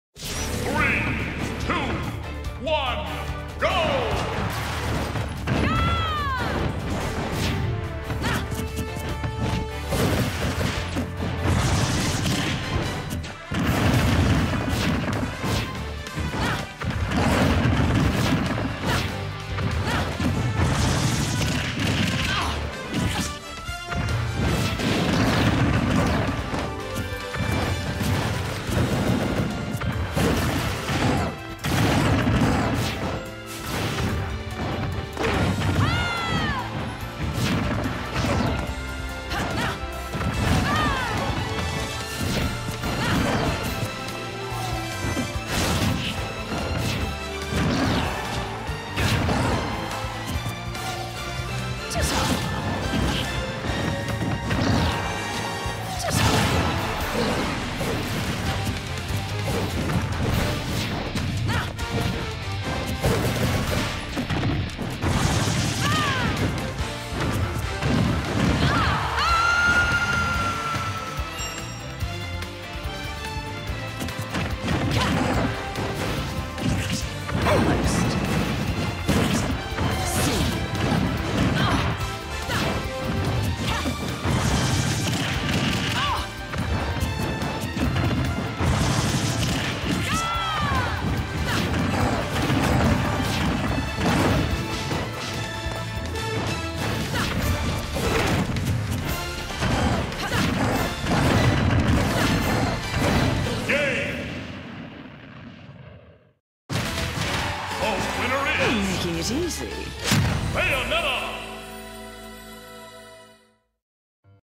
I'm So (NOT) Sorry...
You know, when I first got that jank kill at the end, I felt so bad. However, you proved to be the kind of asshole that deserved it. So, Hero, try to play bracket with that saltmine of an attitude. I'll laugh when your ass gets bodied.